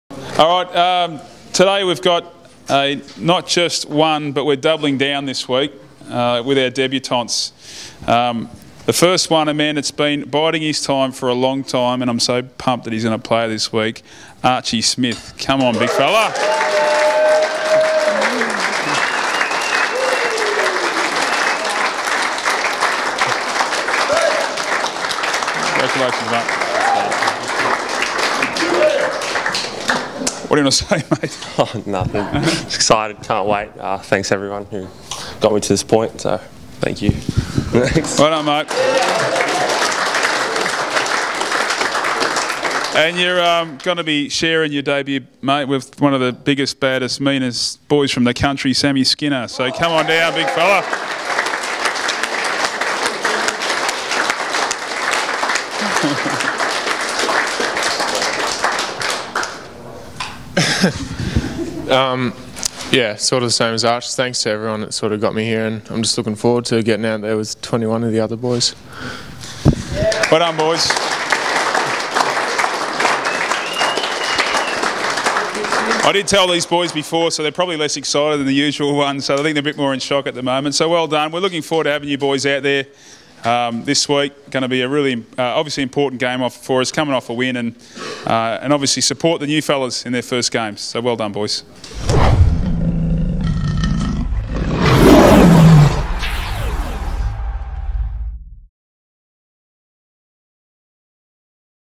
Inside the Den: Skinner and Archie Debut
See the moment Sam Skinner and Archie Smith were told they will be making their debut for the Brisbane Lions.